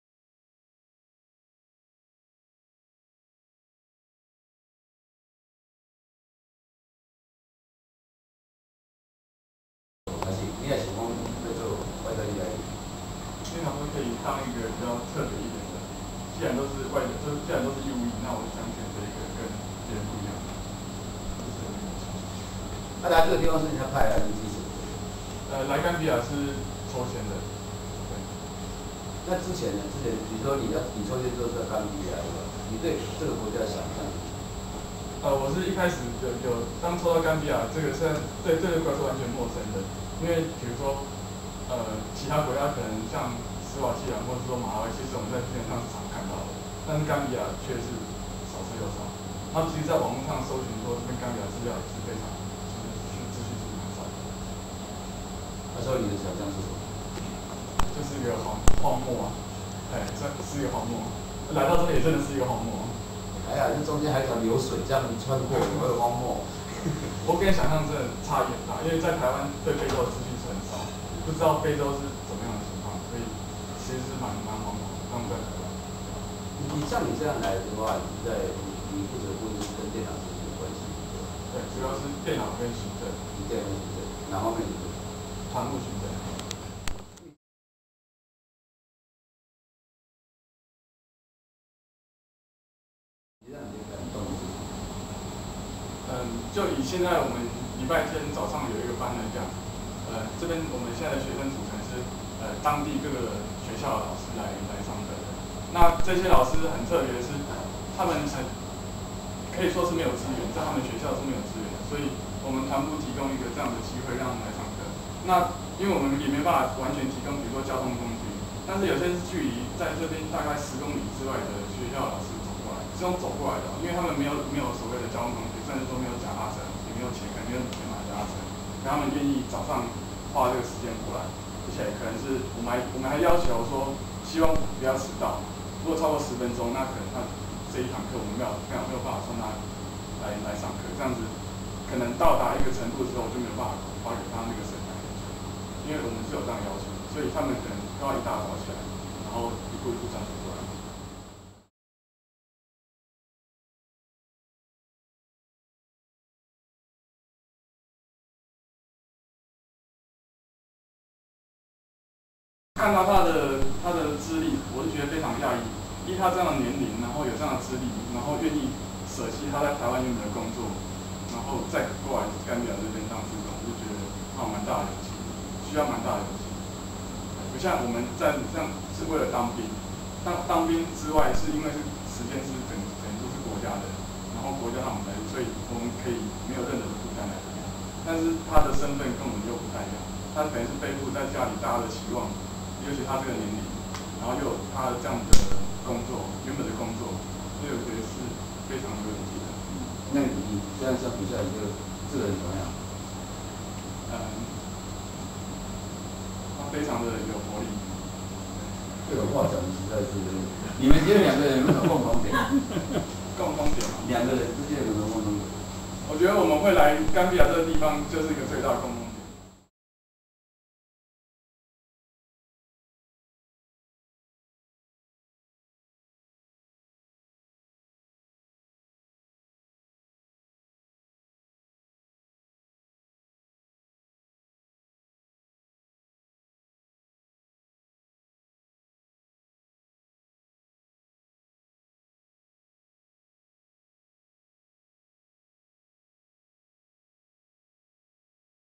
吳念真導演於甘比亞拍攝紀錄片
吳念真導演的團隊在台灣駐甘比亞技術團團部拍攝台灣援外紀錄片 - 專訪替代役男(攝於甘比亞中游省沙普Sapu)